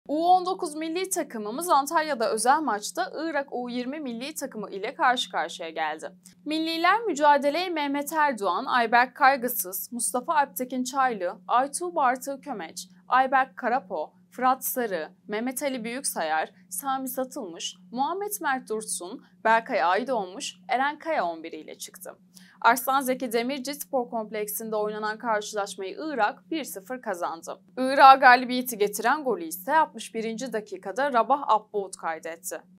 U19 Milli Takımımızdan tatsız prova! | Türkiye U19 0-1 Irak U20
U19 Milli Takımımız, Antalya'da özel maçta Irak U20 Milli Takımı ile karşı karşıya geldi. Milliler mücadeleye, Mehmet Erdoğan, Ayberk Kaygısız, Mustafa Alptekin Çaylı, Aytuğ Batur Kömeç, Ayberk Karapo, Fırat Sarı, Mehmet Ali Büyüksayar, Sami Satılmış, Muhammet Mert Dursun, Berkay Aydoğmuş, Eren Kaya 11'iyle çıktı. Arslan Zeki Demirci Spor Kompleksi'nde oynanan karşılaşmayı Irak 1-0 kazandı. Irak'a galibiyeti getiren golü ise 61. dakikada Rabah Abbood kaydetti.